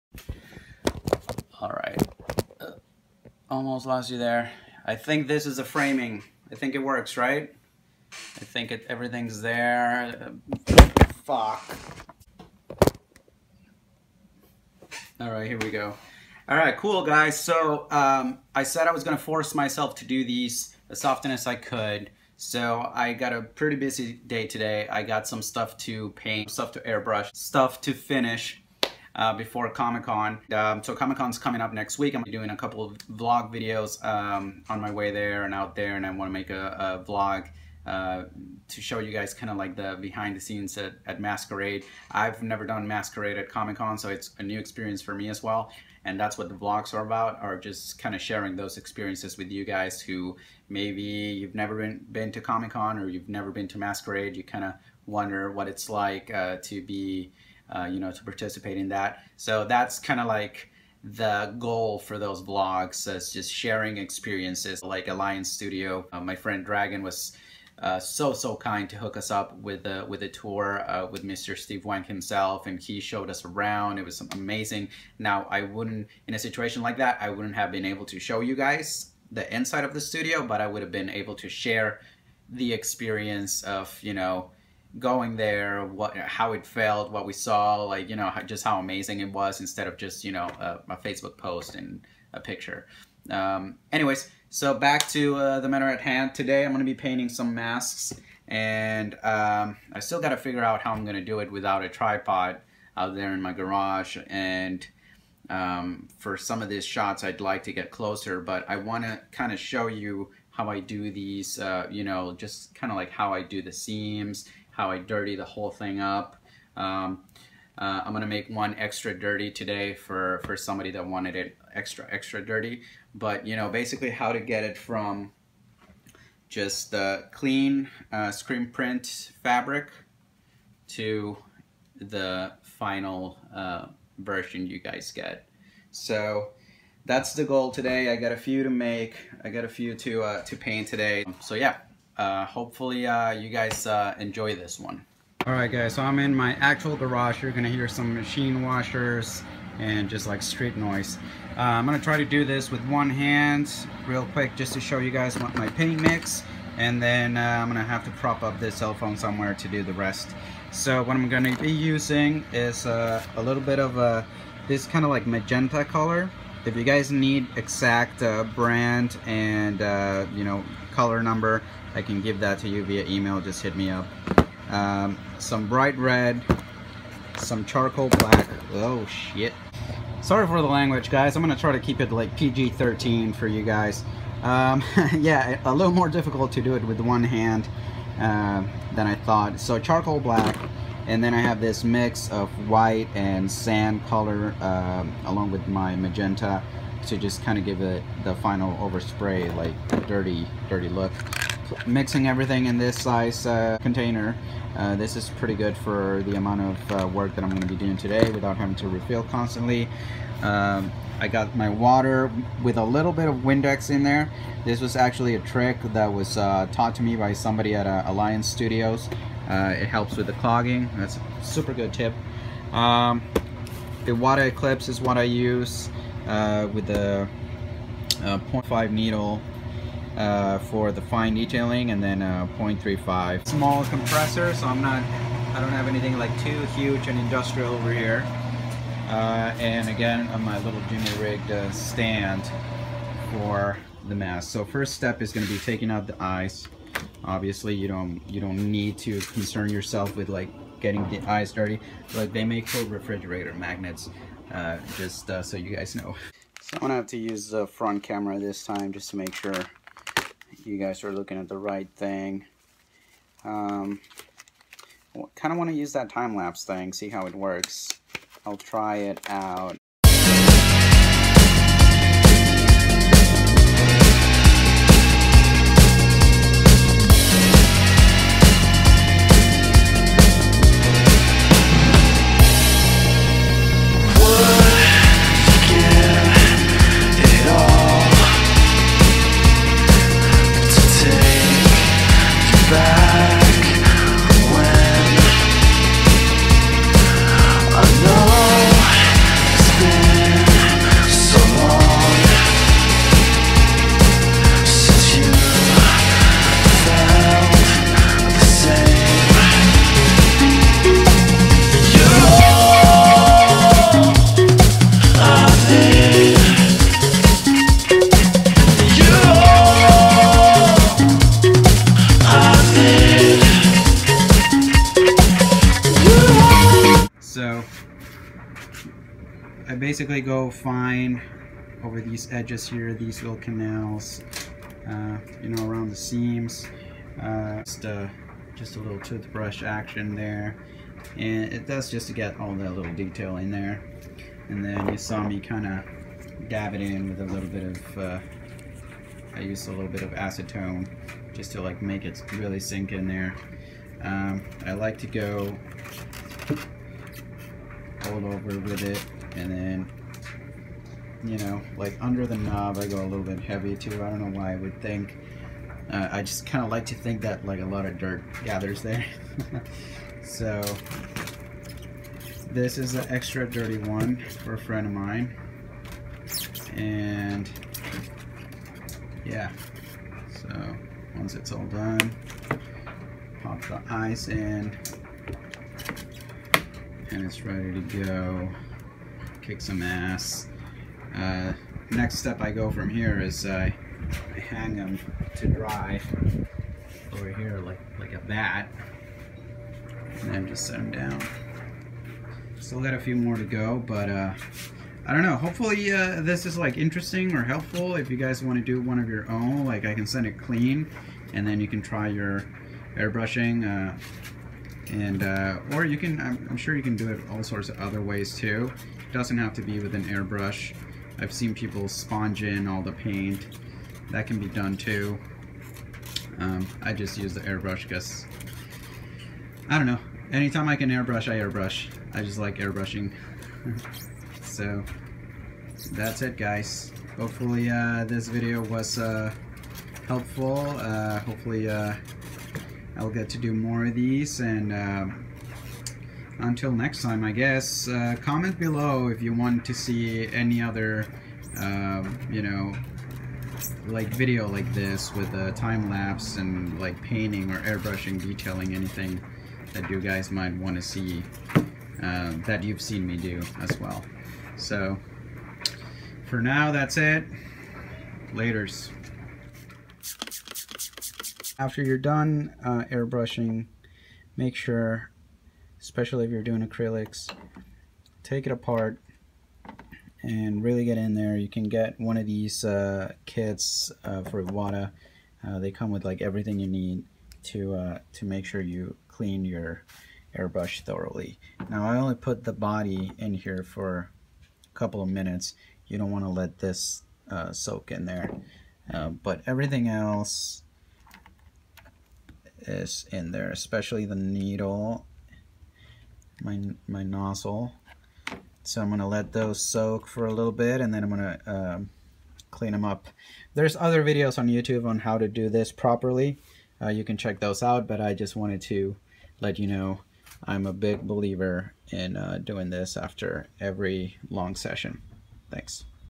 Airbrushing a DP mask
Here's a quick vid on the airbrush weathering of the movie masks.